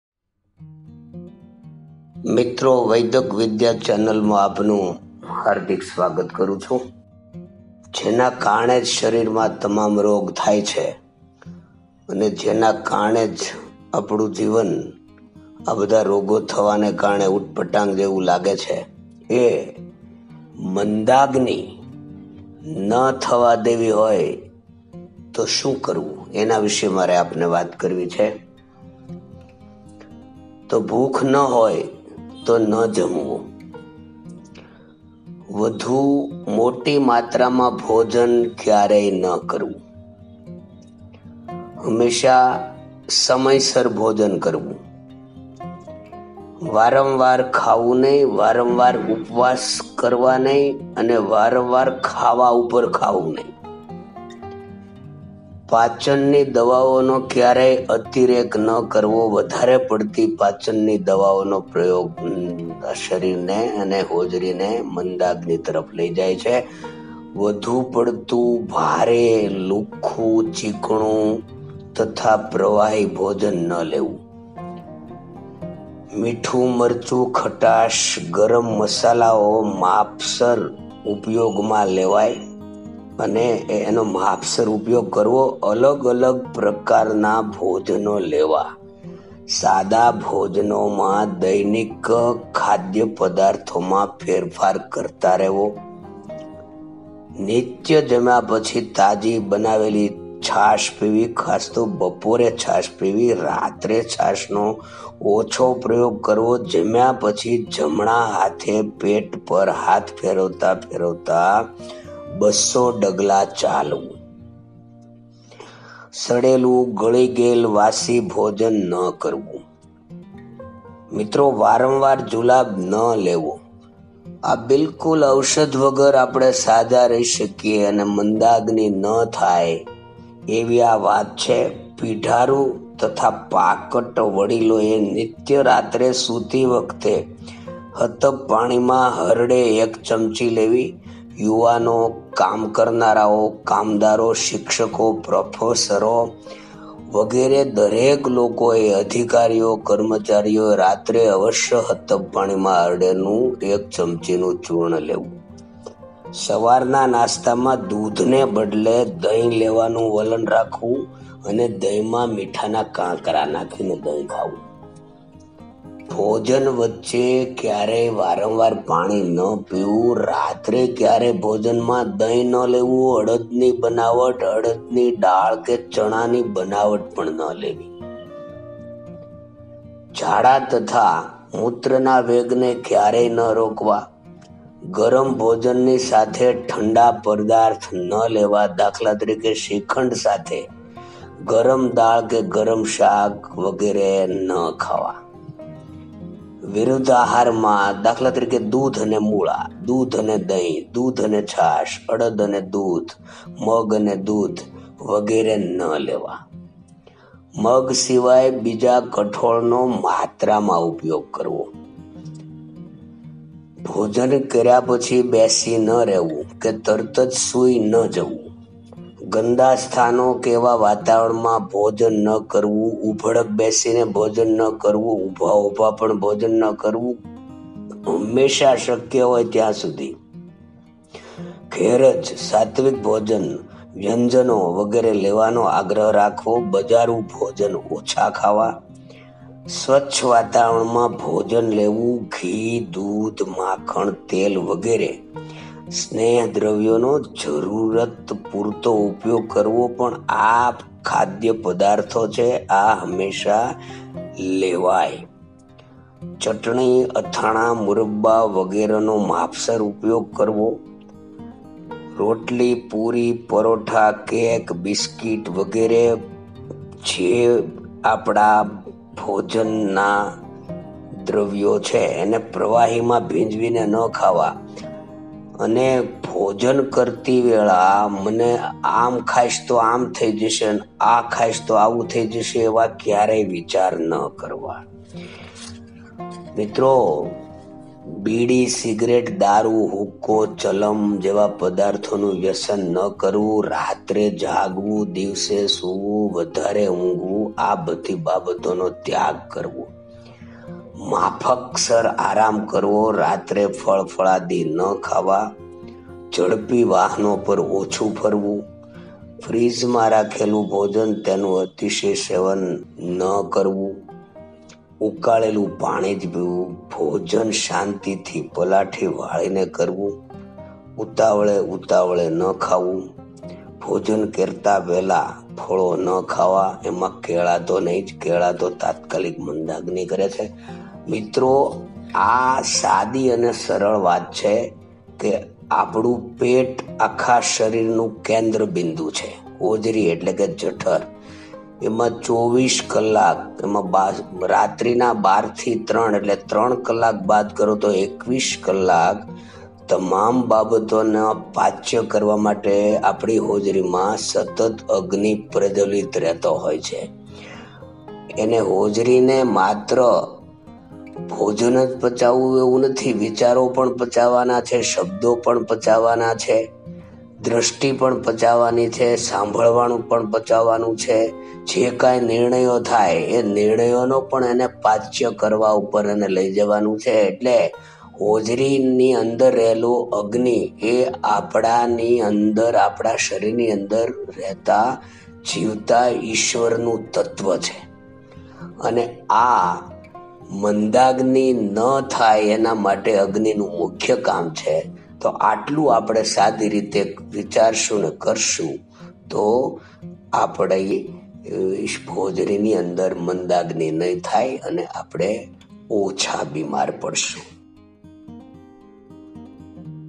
ભૂખ ન લાગવાના કારણો || simply remedy for hungry disease || Part 1
આ વિડિઓ ભૂખ ન લાગવા વિશેનો છે.જે લોકોને ભૂખ ન લાગતી હોય એ લોકો વિડિઓ ખાસ જોજો.ભૂખ ન લાગવાના અનેક કારણો છે.વધુ માહિતી માટે વિડિઓ જુવો.

This video is about not feeling hungry.People who are not feeling hungry should watch the video especially.There are several reasons for not feeling hungry.Watch the video for more information.